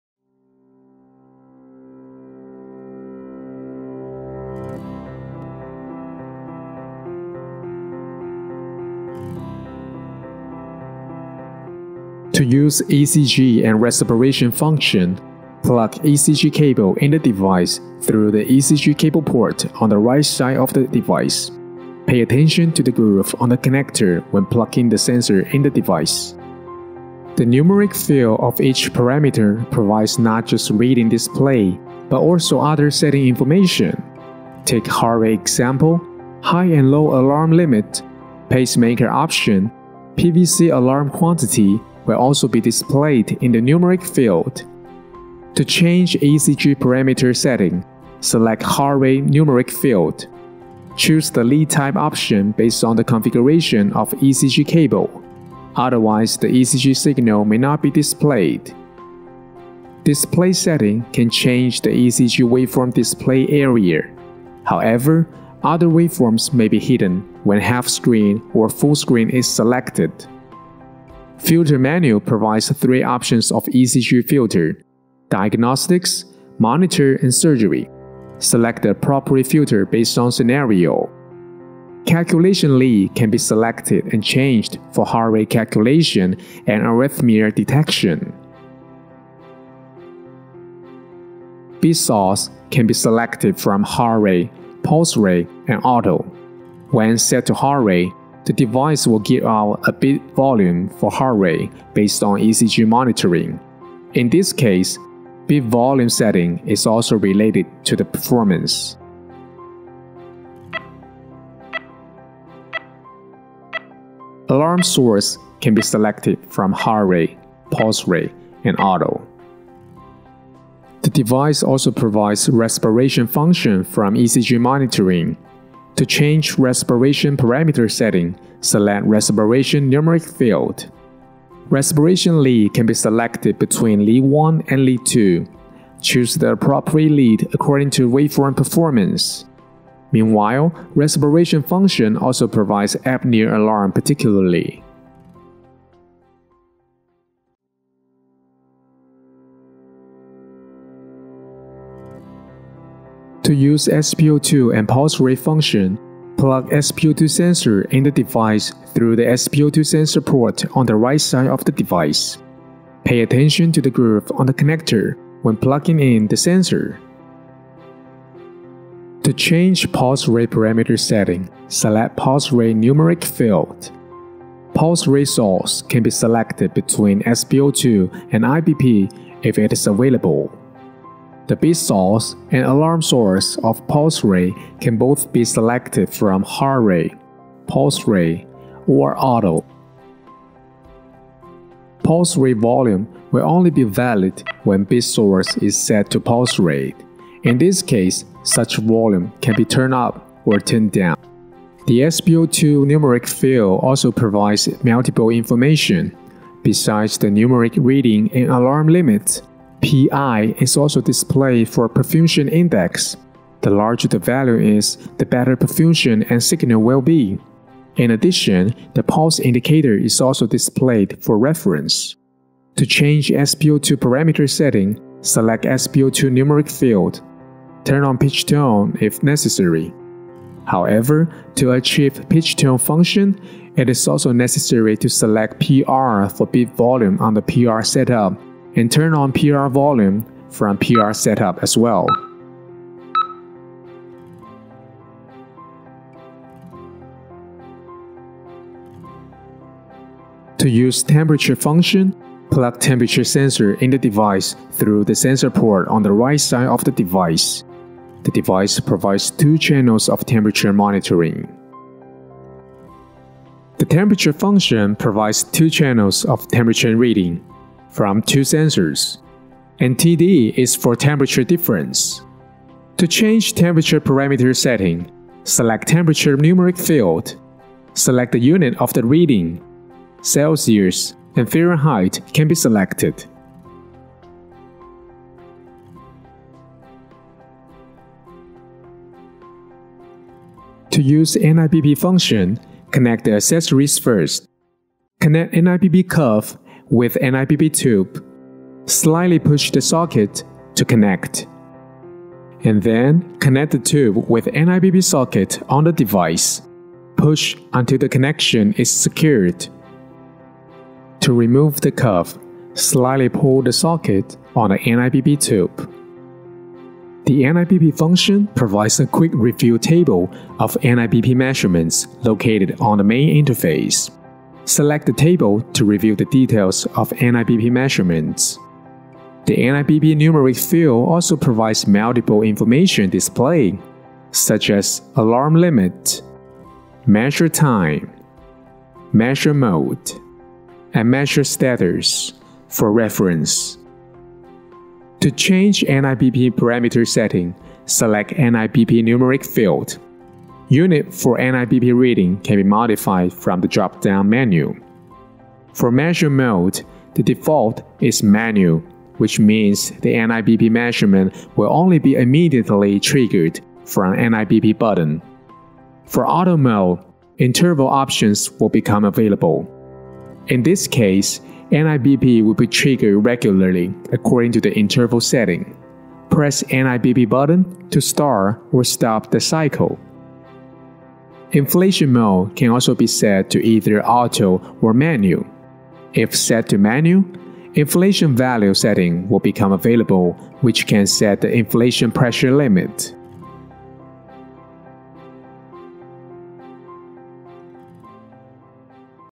Edan iM Series Patient Monitor Operation Guide Parameter Settings
With a wide range of optional parameters from IBP to cardiac output, from capnography to anesthesia gas, the iM series are able to satisfy most clinical demands from general wards to cardiac units, from intensive care units to operation theaters.